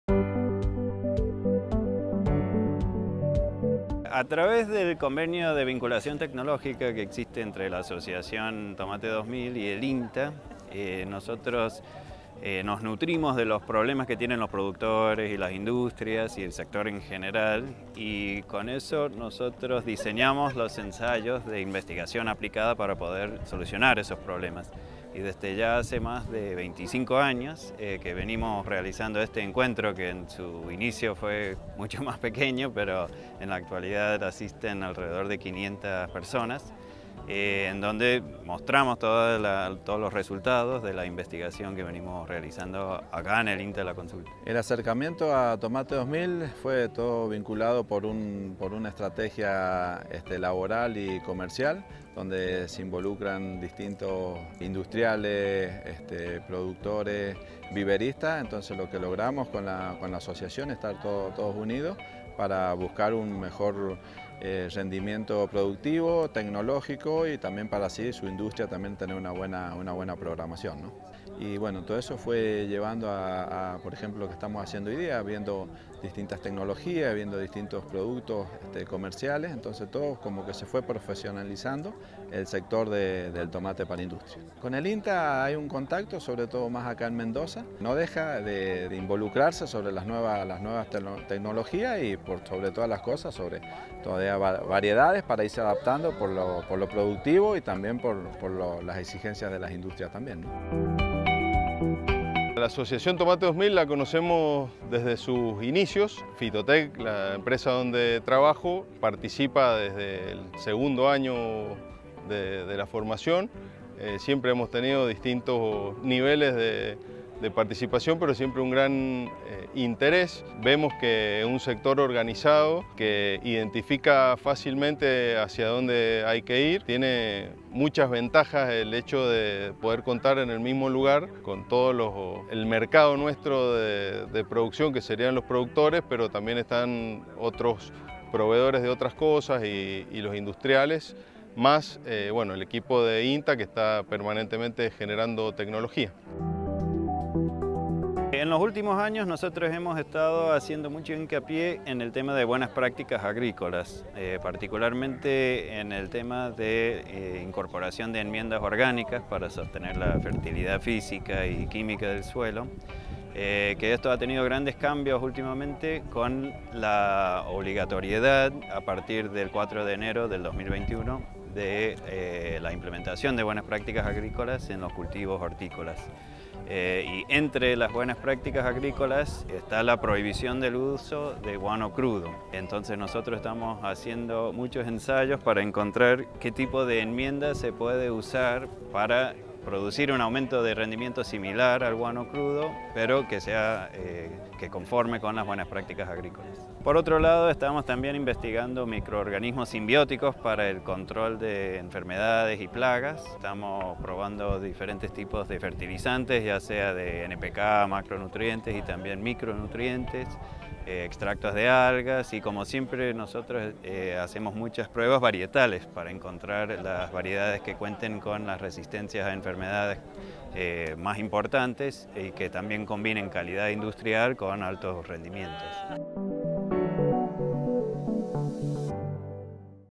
Día de campo de tomate para industria
Con el inicio de la cosecha y un incremento del área cultivada por segundo año consecutivo, se realizó el Día de Campo de Tomate para Industria. La jornada fue organizada en conjunto por la Asociación Tomate 2000 y el INTA La Consulta, en Mendoza.